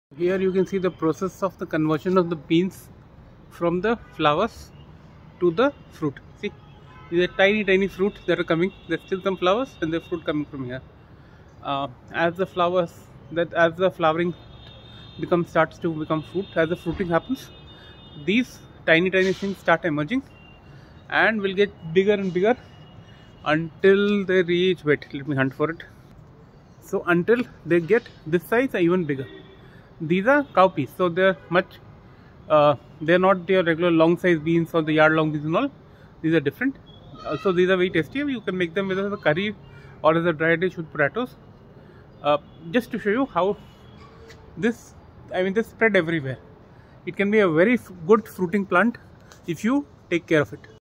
Cowpea - from flower to fruit (including fully developed fruit)
A short video detailing the development of the cowpea plant on my terrace garden. These grow in 5 gallon buckets (and given the previous development, where a lush creeper grows from the bucket, the size of the bucket is adequate). Over the past many years, I have been growing cowpea on the terrace garden and it has been pretty fruitful. There are many varieties of cowpea, and this is the one with the black seeds. The pods make for a great curry or a dry dish with potato.
The flower of the cowpea looks very beautiful, with the purple flowers - as the flower starts to mature, the cowpea starts pushing out from the flower in a small size and then starts to get bigger. It needs to be harvested before the seeds inside become too prominent, else the pod becomes rough and chewy. Many farmers also harvest the pods for the seeds within, letting the pods dry up so that the seeds inside develop fully.
The plant grows in a variety of soils, but does need a good amount of compost or similar additive that helps in producing further. Also, the flowers and pods do attract a lot of pests, so using an organic fertilizer regularly is required.